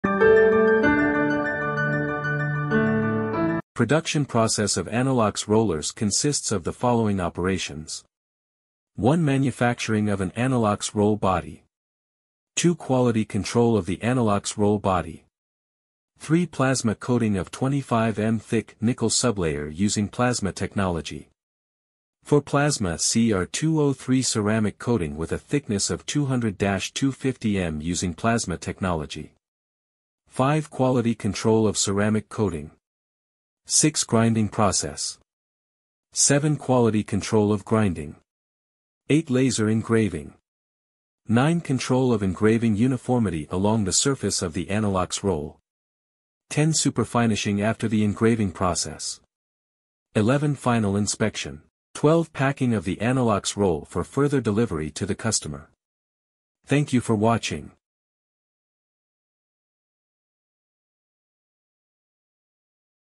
Production Process of Anilox Rollers
Futian Roller is a leading manufacturer of anilox rolls with ceramic coating used in printing industry. The basis of high quality is the comprehensive implementation of all operations: body manufacturing, coating process, grinding process, laser screening and superfinishing.